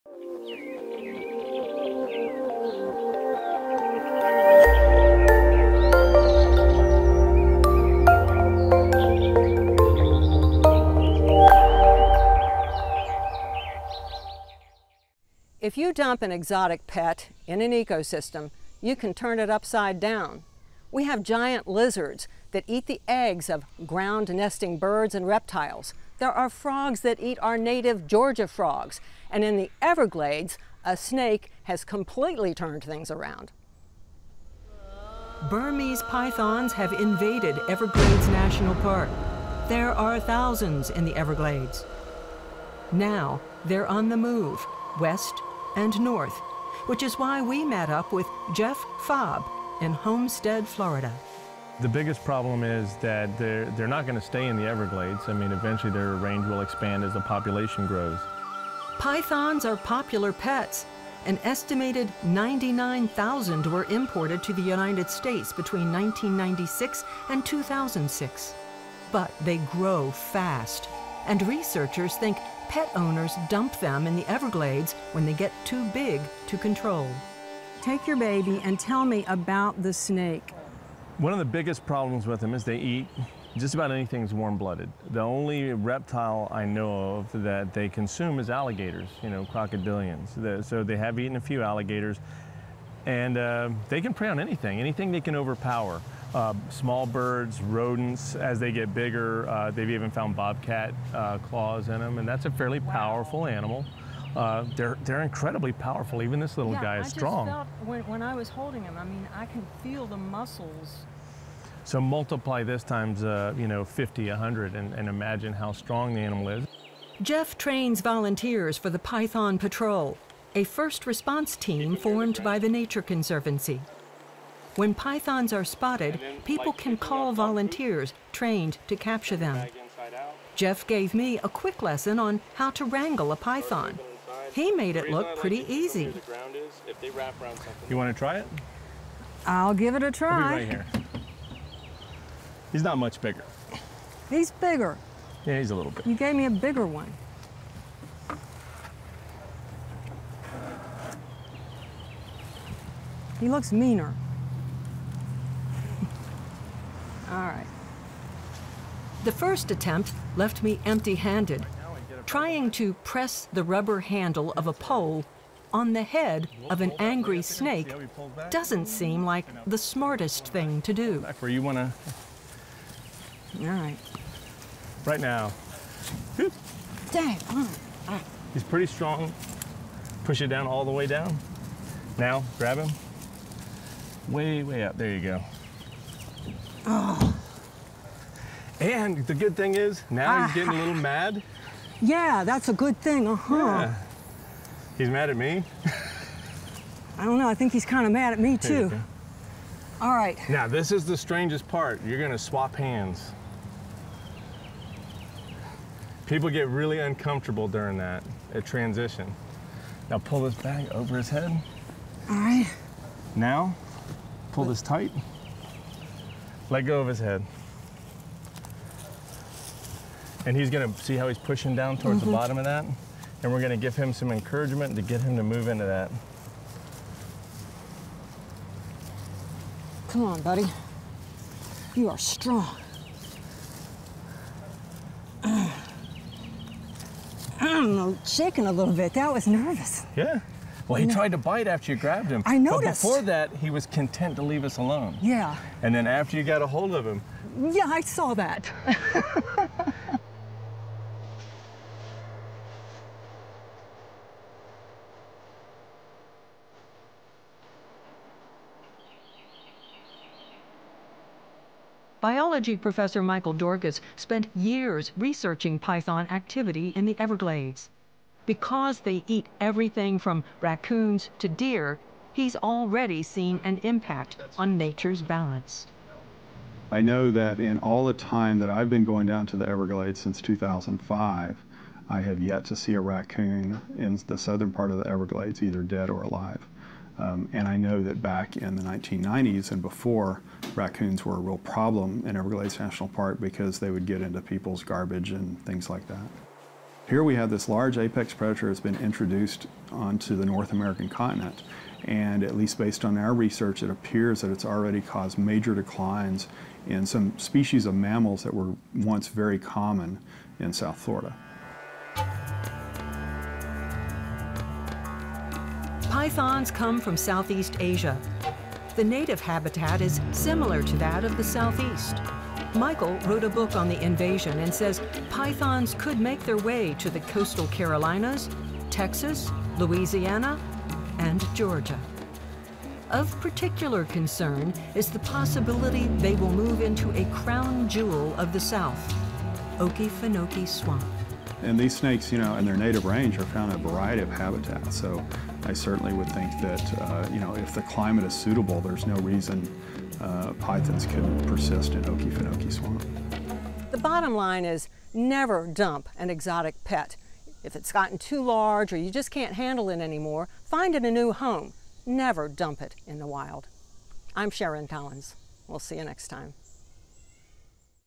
Pythons | Georgia Outdoors' Shorts
Burmese pythons have invaded the Everglades and are on the move north, towards Georgia. Join us as we learn more about these invasive snakes.